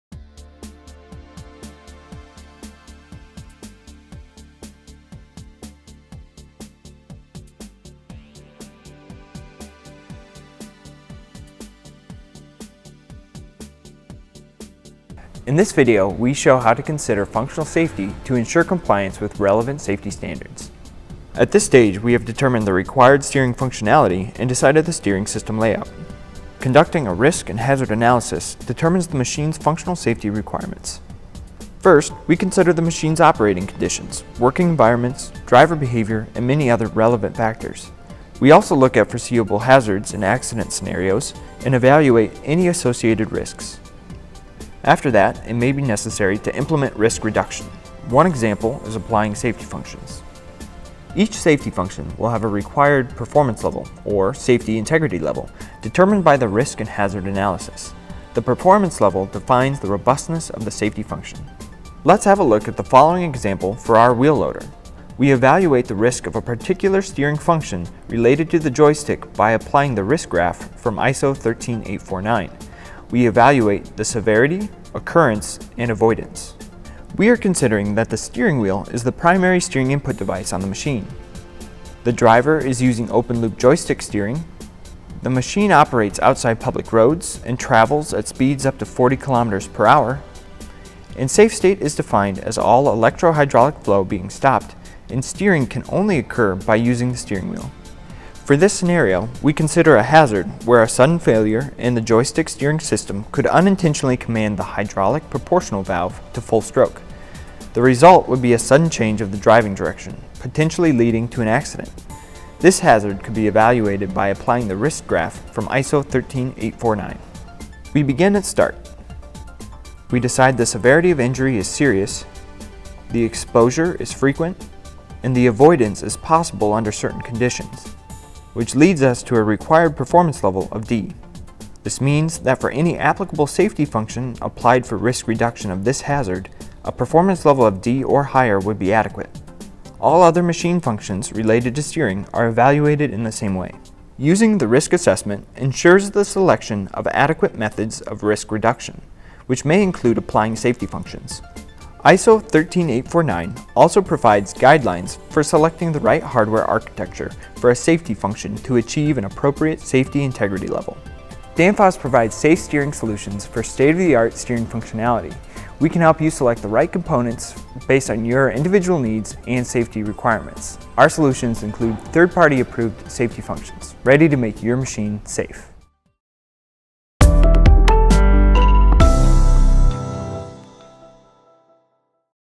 eSteering made easy – Functional safety requirements
Video series: Boost your eSteering system design capabilities:

The video will show you how to consider functional safety of your electro-hydraulic steering system to ensure compliance to relevant vehicle safety standards